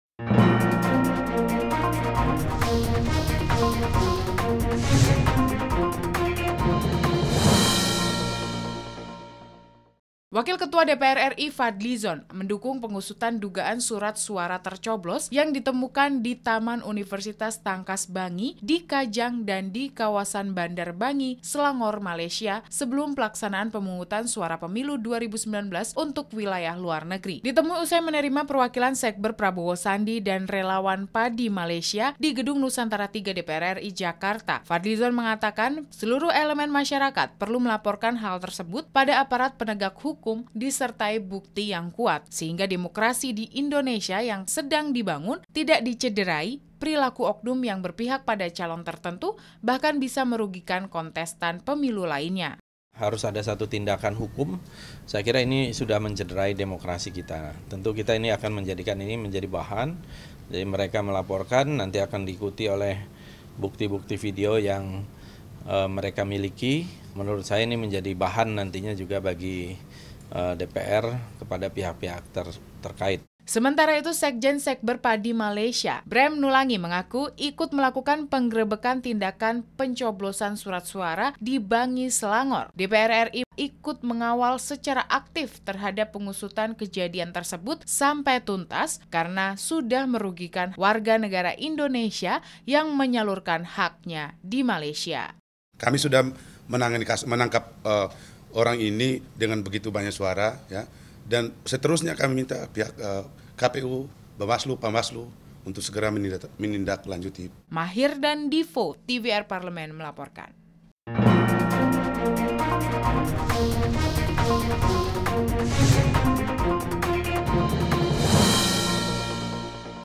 DPR RI - KASUS SURAT SUARA DI MALAYSIA HARUS DIUSUT TUNTAS
// WAKIL KETUA DPR RI FADLI ZON MENDUKUNG PENGUSUTAN DUGAAN SURAT SUARA TERCOBLOS YANG DITEMUKAN DI TAMAN UNIVERSITAS TANGKAS BANGI DI KAJANG / DAN DI KAWASAN BANDAR BANGI, SELANGOR, MALAYSIA / SEBELUM PELAKSANAAN PEMUNGUTAN SUARA PEMILU 2019 UNTUK WILAYAH LUAR NEGERI // PENCOBLOSAN KEJADIAN TERSEBUT DINILAI SEBAGAI SUATU KEJAHATAN YANG MENCIDERAI PROSES DEMOKRASI INDONESIA //

NARSUM :
FADLI ZON / Wakil Ketua DPR RI / KORPOLKAM / FPGERINDRA / Dapil JABAR V
BREM NULANGI / SEKJEN SEKBER PADI